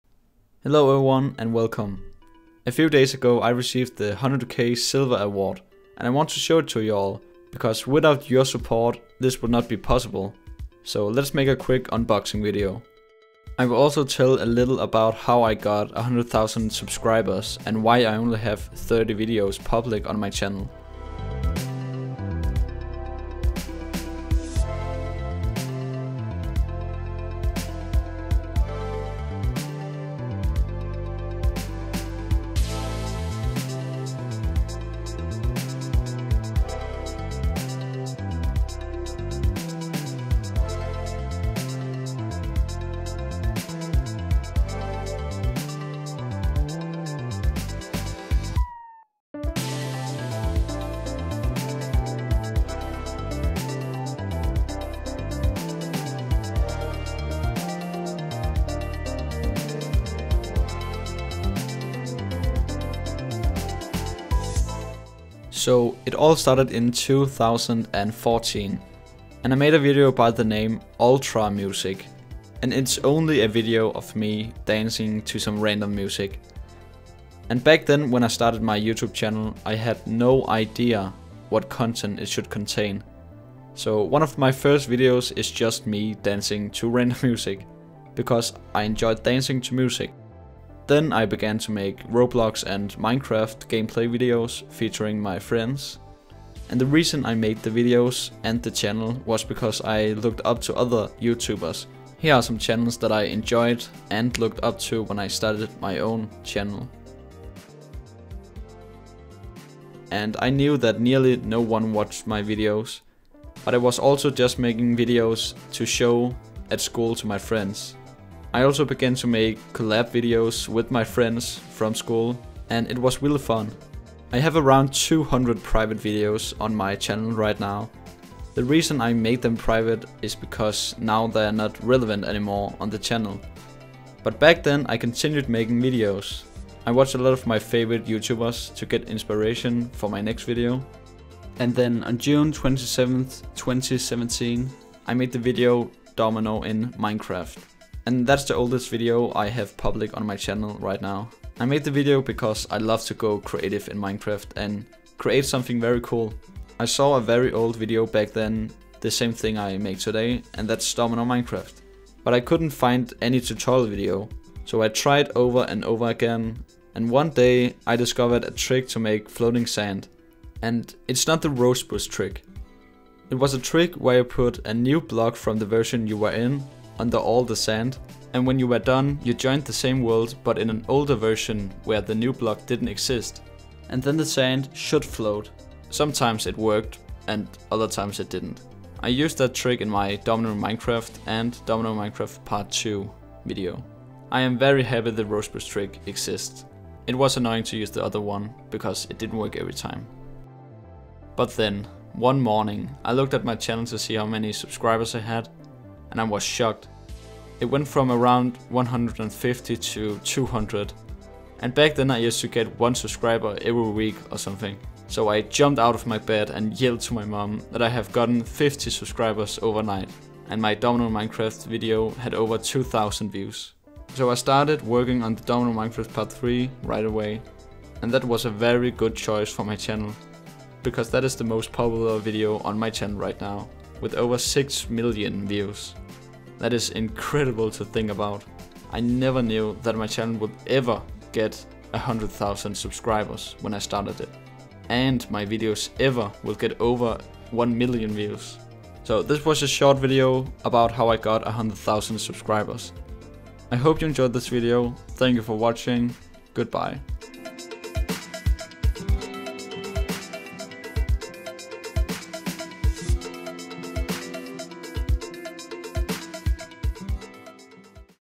100K Silver Award Unboxing! + How I Got 100K Subscribers
In this video, I will unbox the silver award and tell about how my channel started growing so fast.